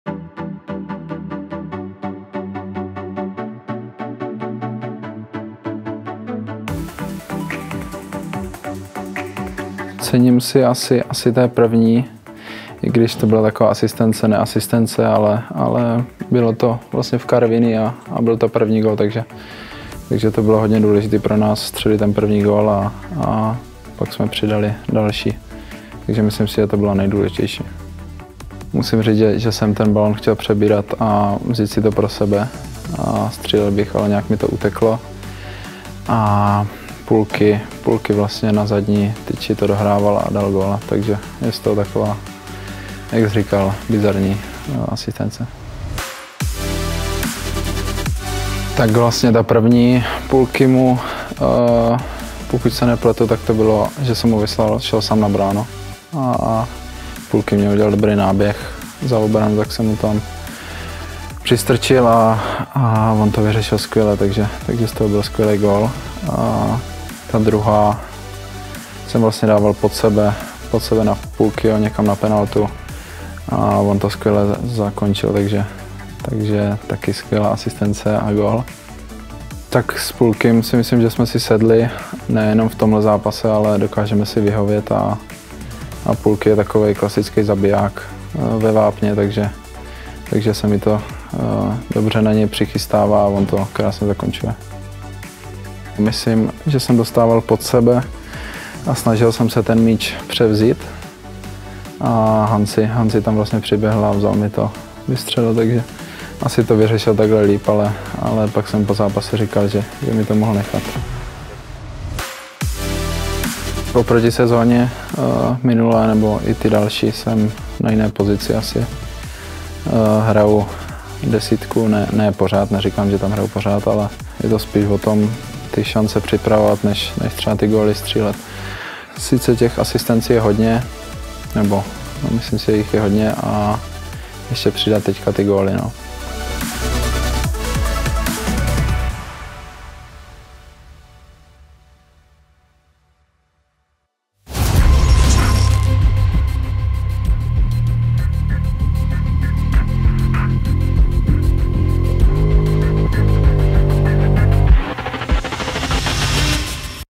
Přihraj:Král asistencí - Adam Hložek
Devatenáctiletý letenský talent se stal čtvrtým Přihraj:Králem asistencí letošní sezóny 👑 V rozhovoru popisuje nejen své gólové přihrávky ale také to, jak vnímá svou novou roli v týmu 🎥⏯